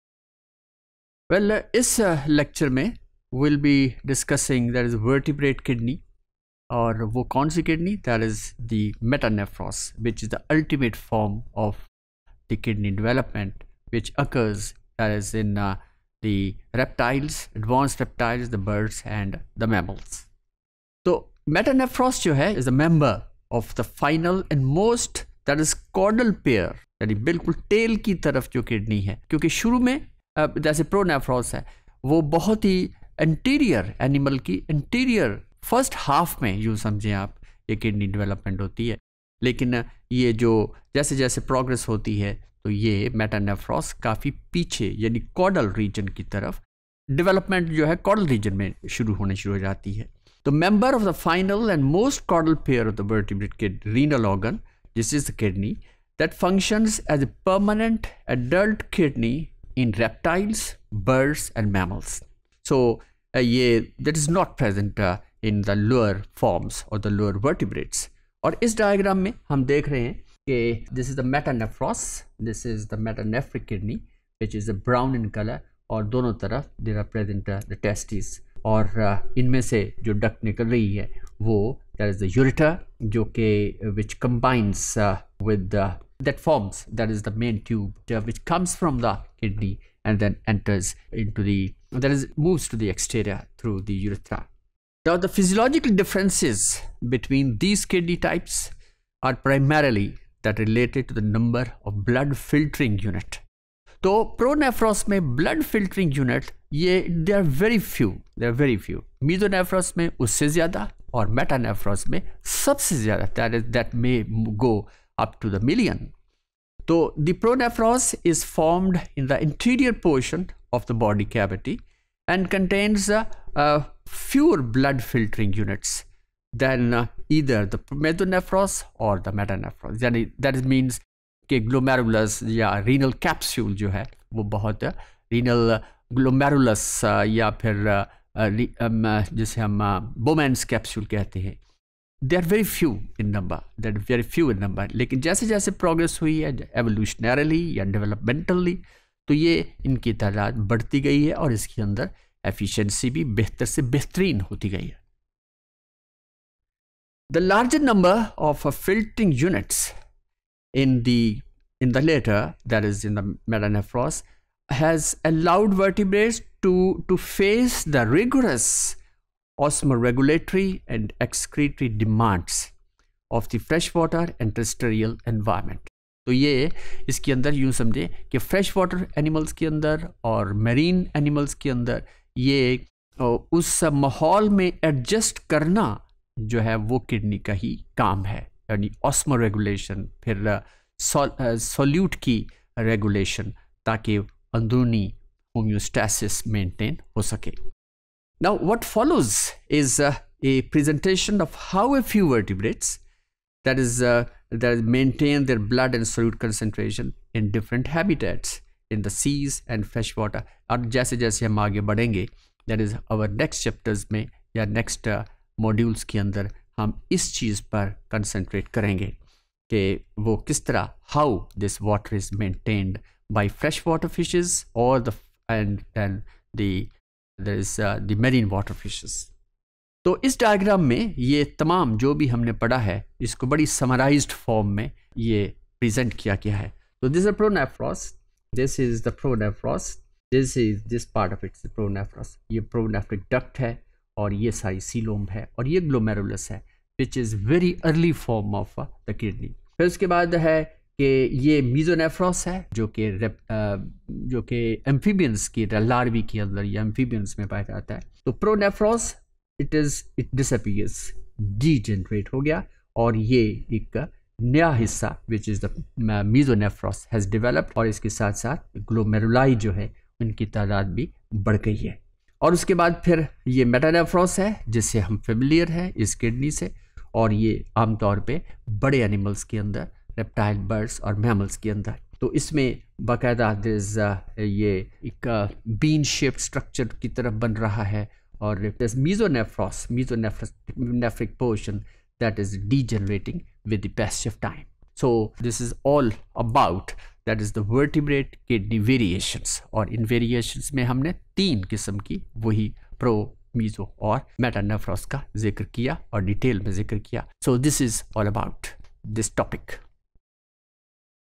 ZOO302_Topic095
ZOO302 - Animal Form & Function-II
Vertebrate Kidney Variations Metanephros
By Dr. Javaid Iqbal